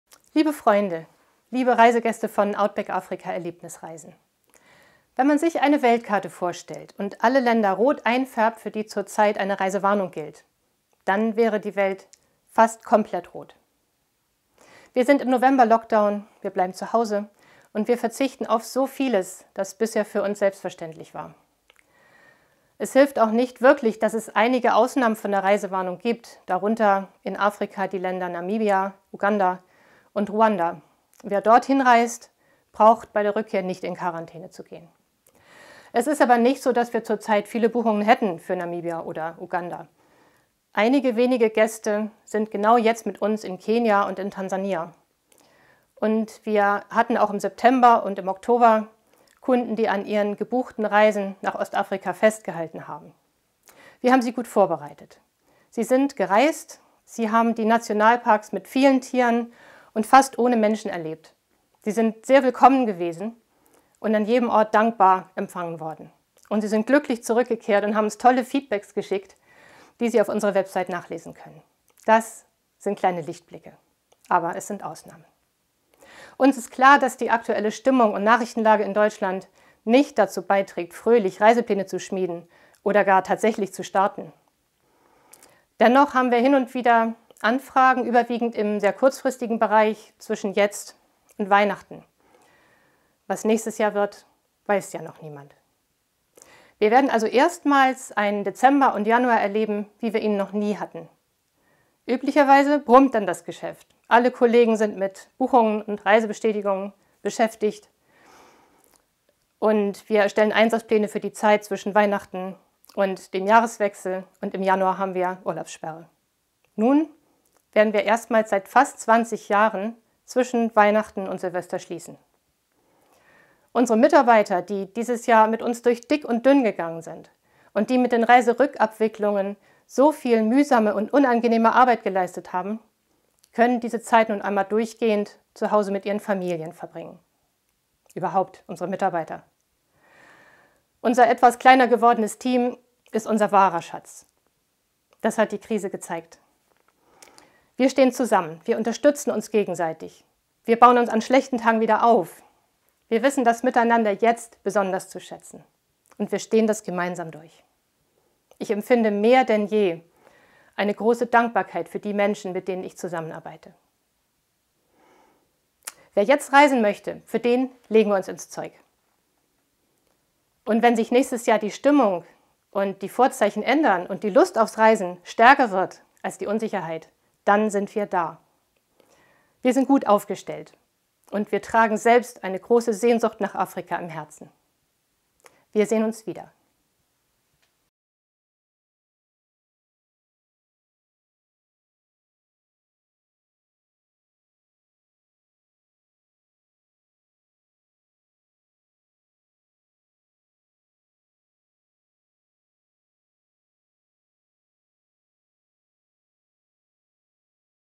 Svenjas Zeug, November 2020
Was sonst noch geschah, nicht immer nur in Afrika - Svenjas Zeug, die Video-Kolumne der Outback Africa Chefin. Diesmal berichtet sie von den ersten Kunden, die wieder nach Afrika reisen, vom Büro-Team und blickt voraus auf Weihnachtsurlaub, der länger ausfallen wird als in den vergangenen 19 Jahren.